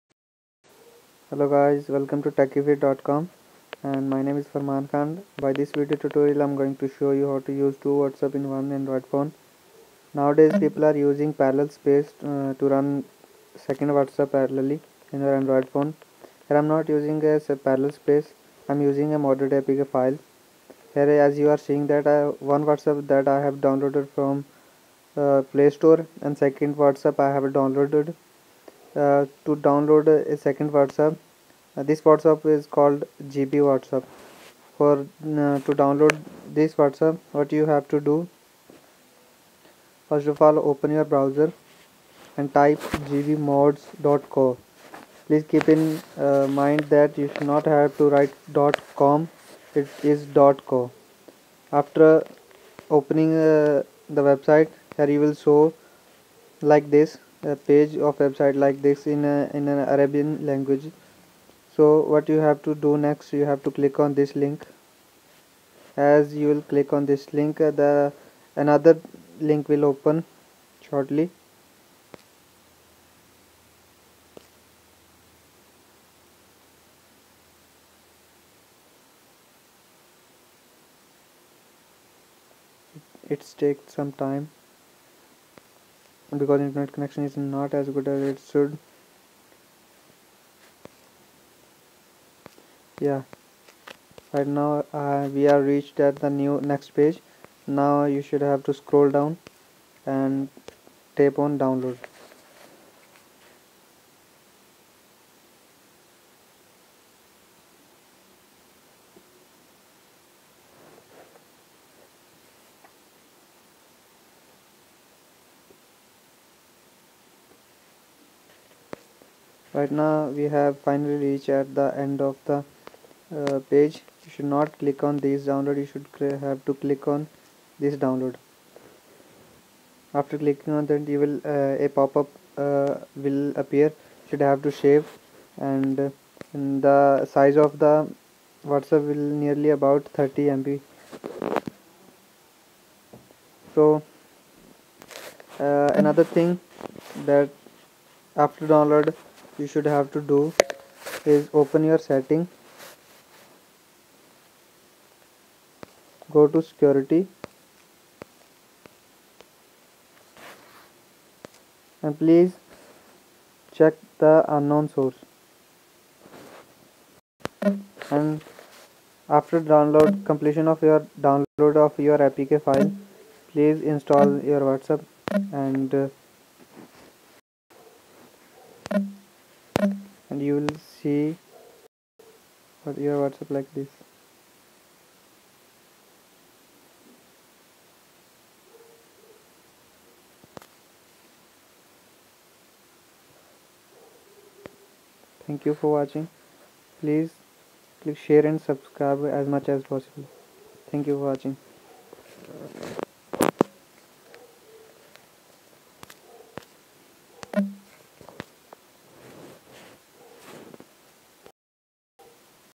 How To Use Two Whatsapp In One Android Phone Without Parallel Space
Moded Apk File Of Whatsapp Like gbwhatsapp and Whatsapp+